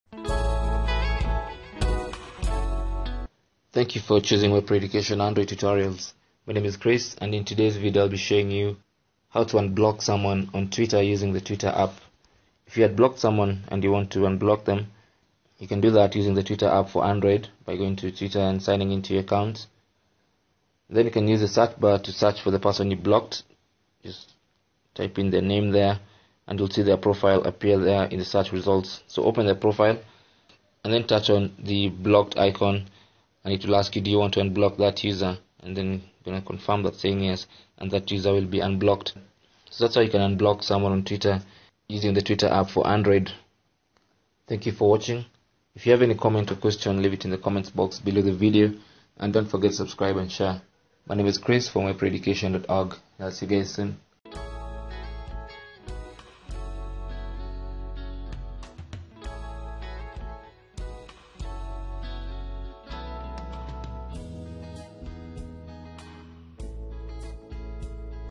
How To Unblock Someone On Twitter [Android]
You can unblock someone on Twitter using the Twitter app on your Android device.

To unblock someone you had previously blocked,  you need to go to the person's profile and unblock them from there.

Use the search bar to find the person you blocked.  Open their profile page.  If that user is blocked,  you will see a 'Blocked'  icon besides the name.
Tap on that icon and confirm you want to unblock that user.